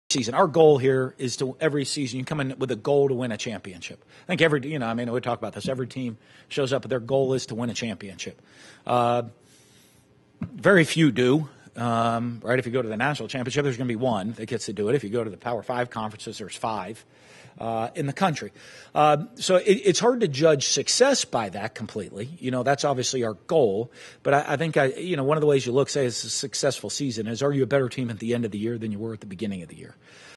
Mullen Presser after firing Grantham
Mullen explains goals after firing Grantham... WHAT?